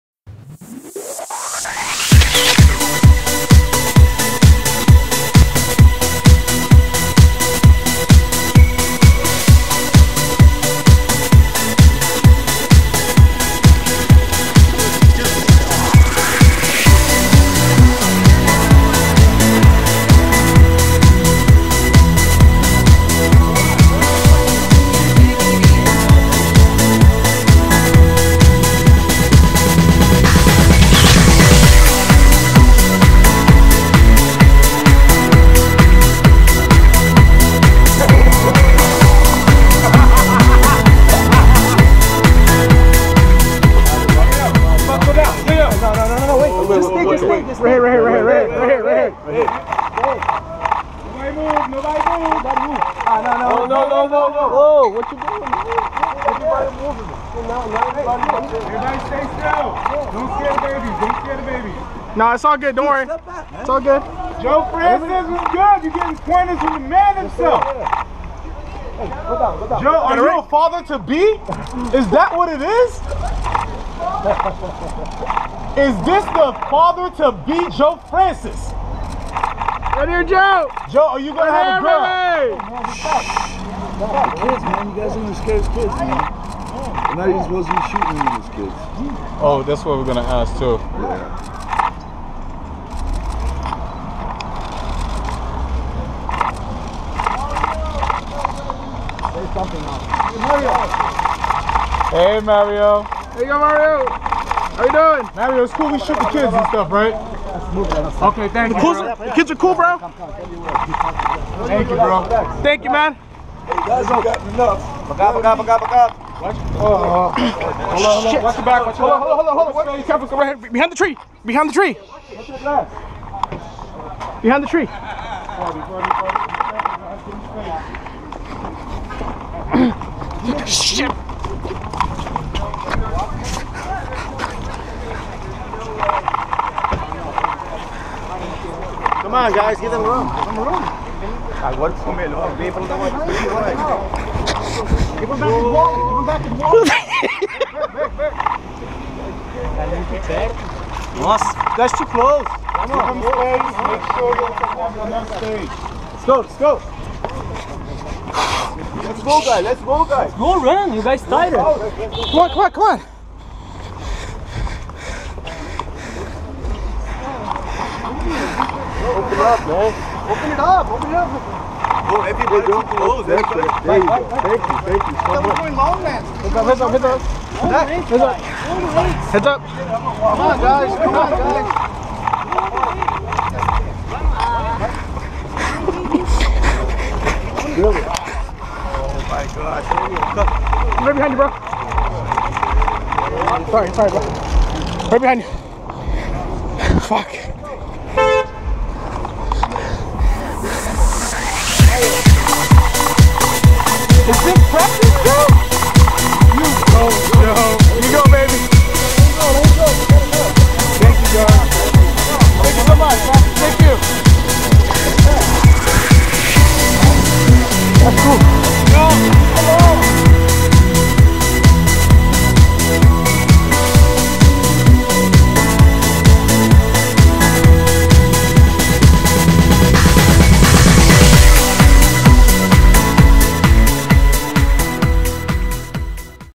La Paparazzi - Mario Lopez and Joe Francis spent the afternoon shopping with family!
Mario Lopez and Joe Francis Spend the afternoon Shopping with family! Joe Francis the CEO of Girls Gone Wild has been recently released from Prison after a brief year sentence! Joe and Mario were spotted in West Hollywood shopping with family! Take a Look!